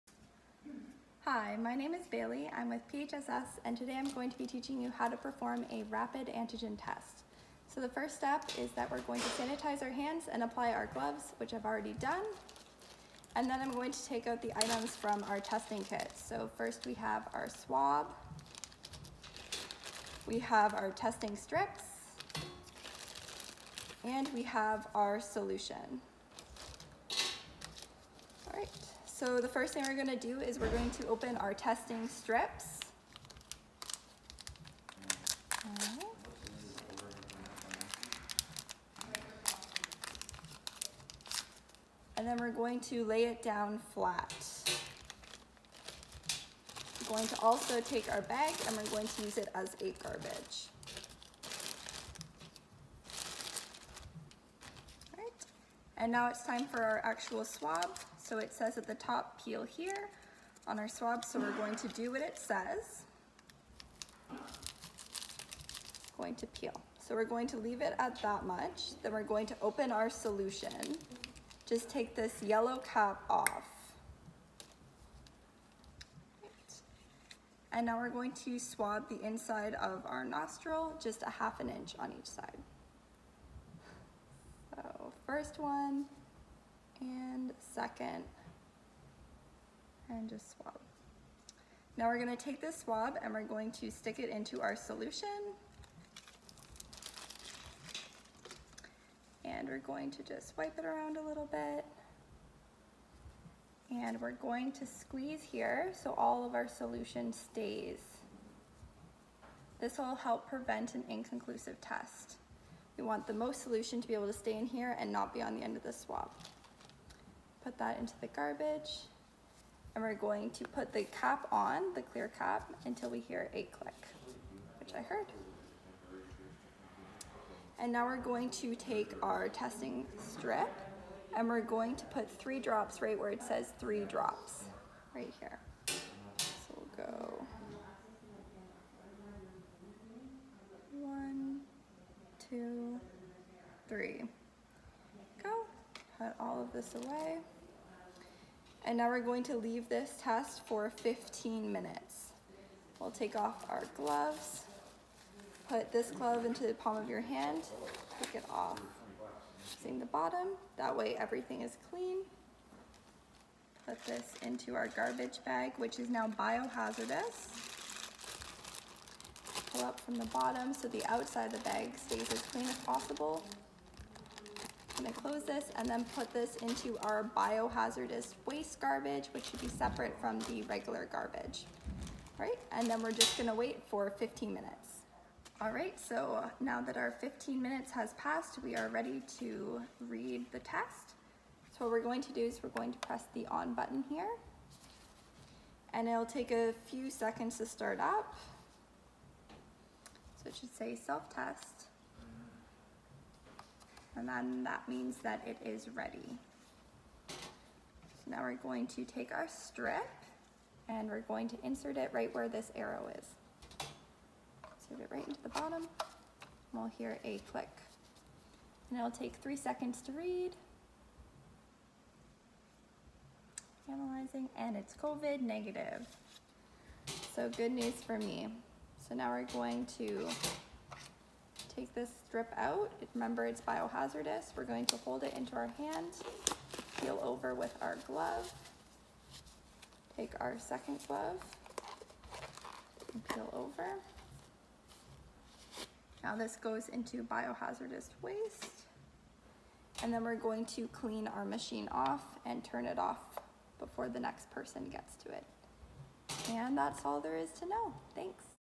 Rapid Antigen Testing: BD Veritor self test tutorial
Property of PHSS: Demonstrated by Bailey Fulton BScN